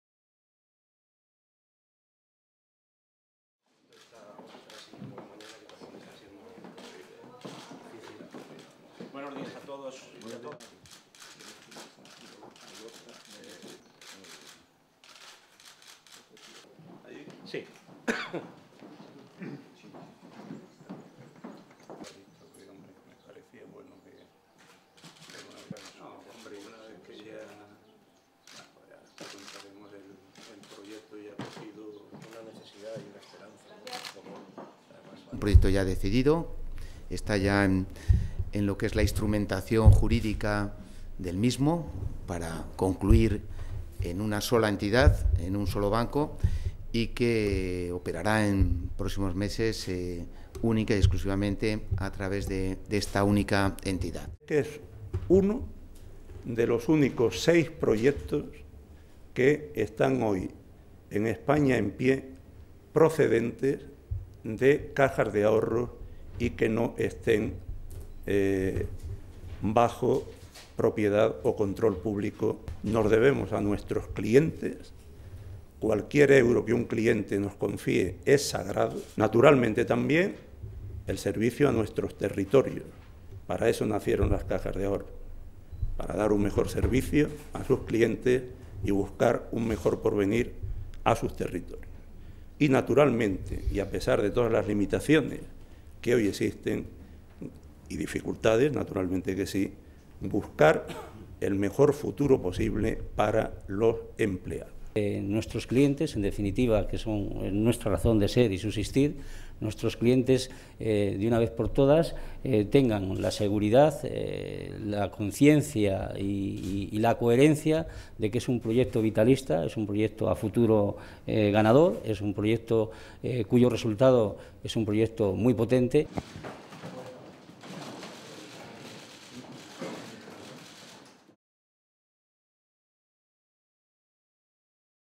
Caja España-Duero y Unicaja ratifican el compromiso con Castilla y León
La reunión mantenida entre el presidente de la Junta de Castilla y León, Juan Vicente Herrera y los presidentes de Unicaja, Braulio Medel, y Caja España-Duero, Evaristo del Canto, ha dado como resultado el compromiso de ambas las entidades con los intereses del territorio de Castilla y León, en el que han reconocido la construcción de un espíritu de colaboración y confianza mutua, imprescindible para el desarrollo de la integración